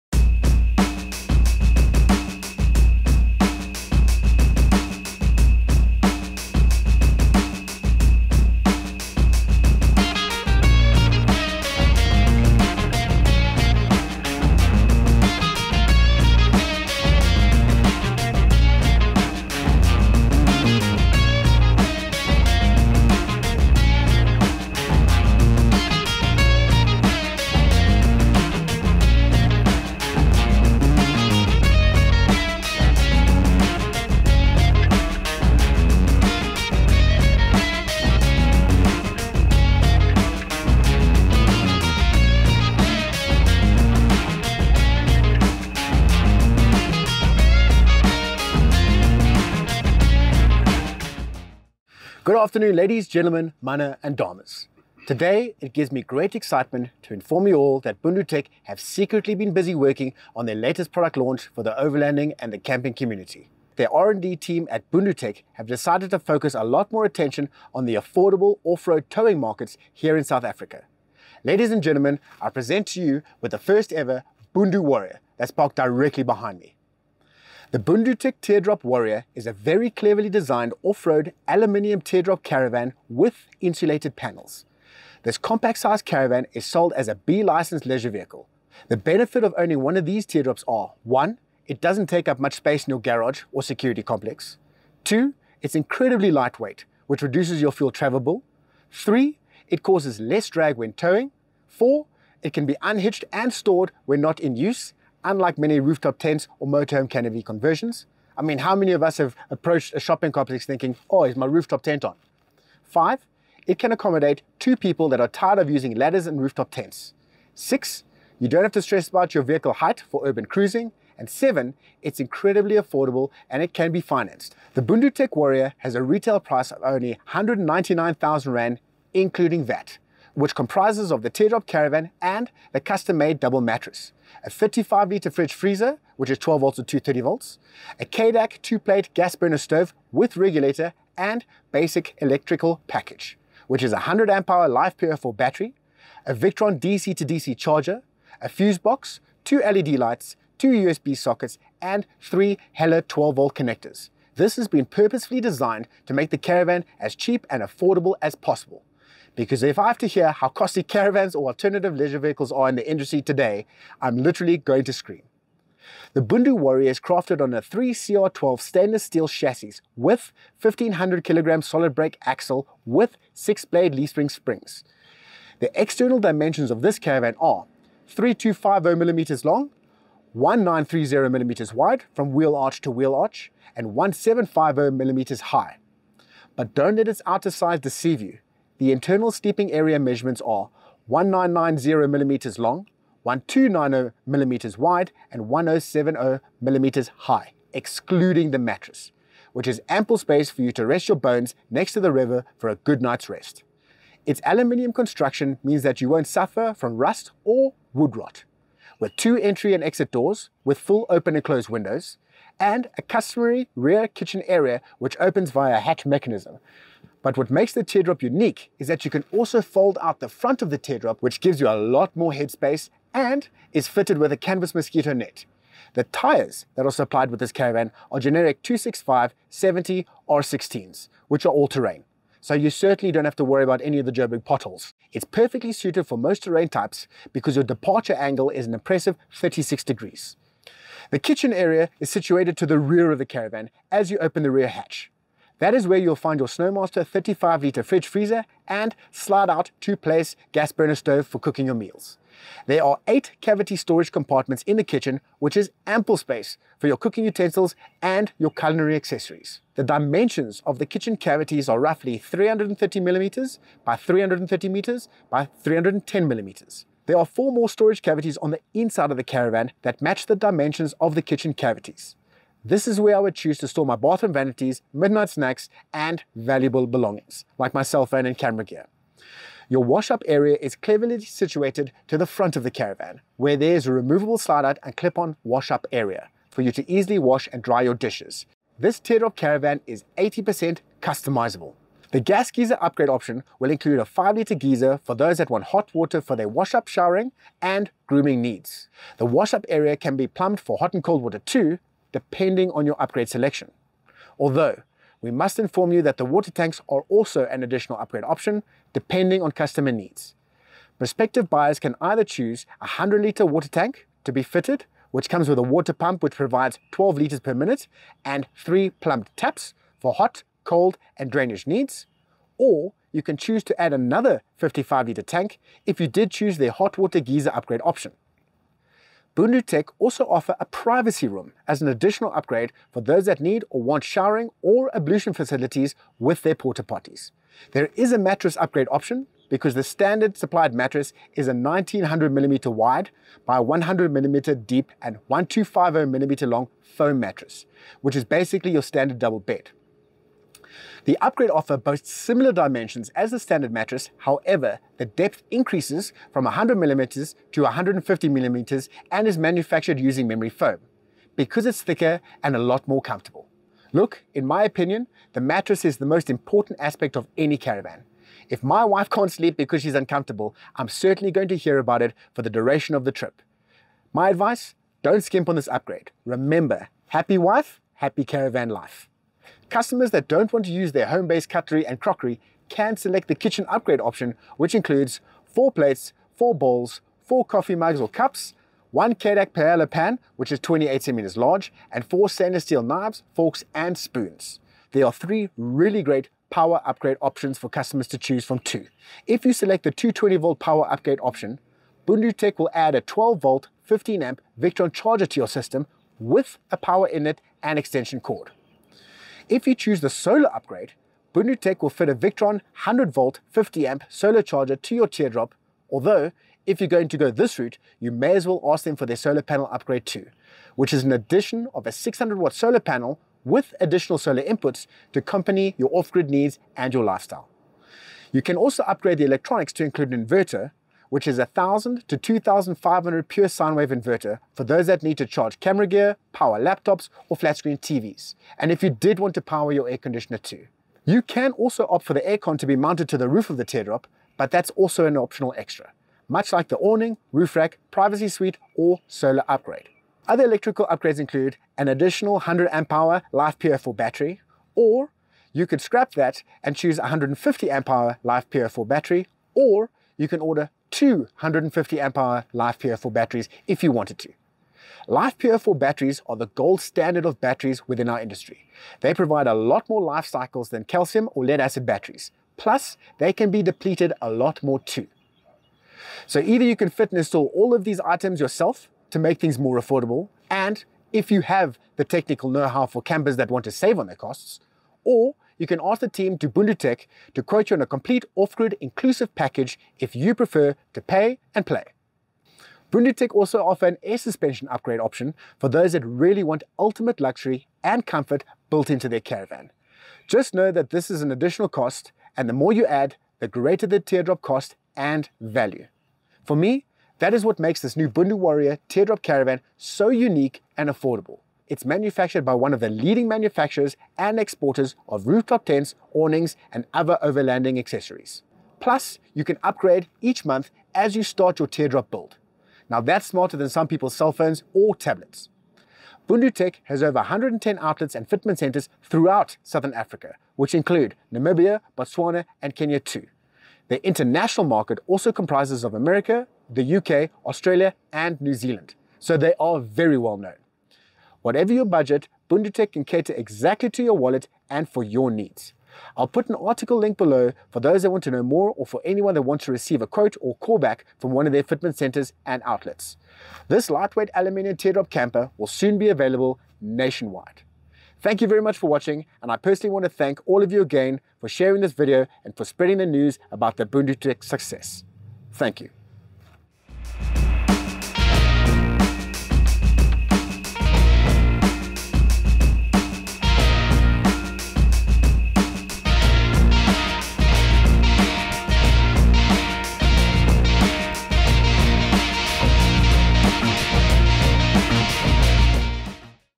Bundutec Launches New BunduWarrior, A B-License Overlanding & Customisable teardrop Caravan [Review]
The Bundutec Warrior is a very cleverly designed off-road aluminum teardrop caravan with insulated panels for any climate.

This compact sized teardrop caravan is sold as a B licence leisure vehicle (which means no further personal licence application or upgrading are required). Remember, if you tow a leisure vehicle or trailer greater than 750KG you will need a Special EB licence – as in the case of their BunduTRAIL.

With their new Warrior teardrop caravan – anyone can purchase one or legally tow it. Provided that you do not exceed your GVM. Which is 750Kgs.

The benefits to owning one of these new BunduWarrior teardrop caravans are:
1. It doesn’t take up much space in your garage or security complex.
2. Its incredibly lightweight which reduces your travel fuel bill.
3. It causes less drag when towing.
4. It can be unhitched and stored when not in use- unlike many RTT’s or motorhome camper conversions.
5. It can accommodate 2 people that are tired of using ladders and rooftop tents
6. You don’t have to stress about your vehicle height for urban cruising.
7. Its incredibly affordable AND it can be financed – unlike other Rooftop tents.
8. Its aluminum construction means that you won’t suffer from rust or wood rot.
9. This teardrop is 80% customizable!